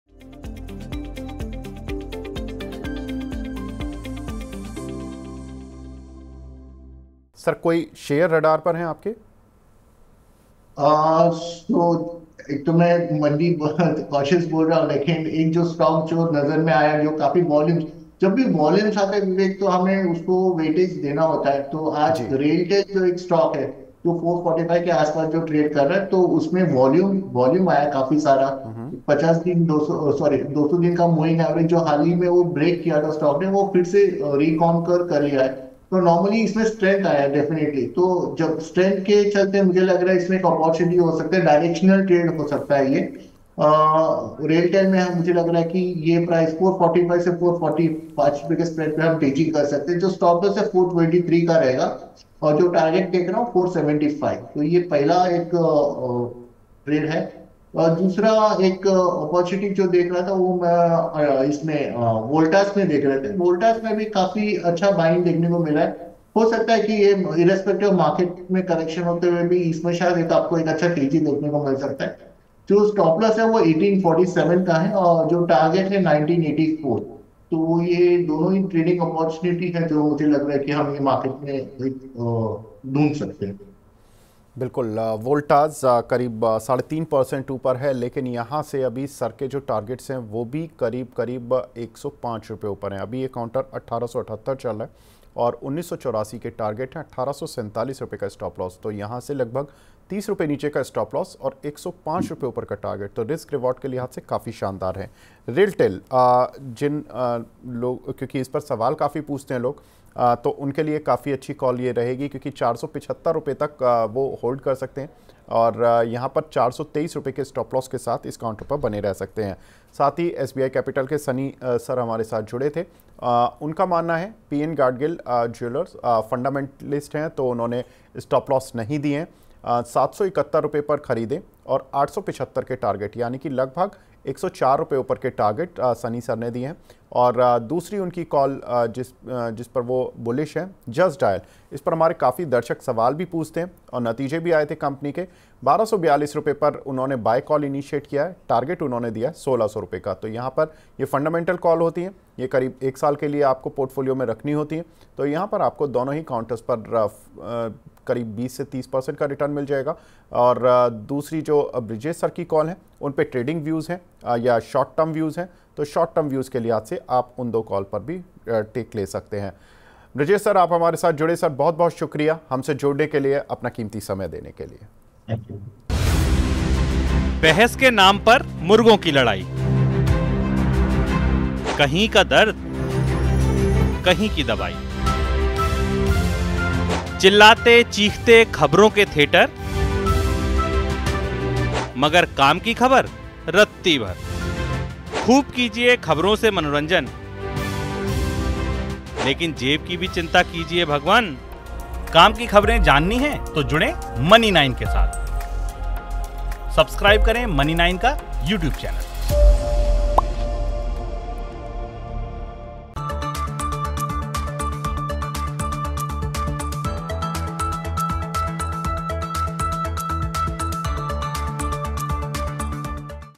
PNG Jewellers, Just Dial, Voltas, Railtel Share में क्या करें? Stocks To Buy
Stocks To Buy :  लंबी अवधि के लिए कहां बन रहे निवेश के मौके? किन शेयरों में बनेगा लॉन्ग टर्म में पैसा? कौन सा रेलवे शेयर है निवेश के लिए बेहतर? छोटी अवधि के लिए किन शेयरों में करें निवेश? जानने के लिए देखिए ये वीडियो.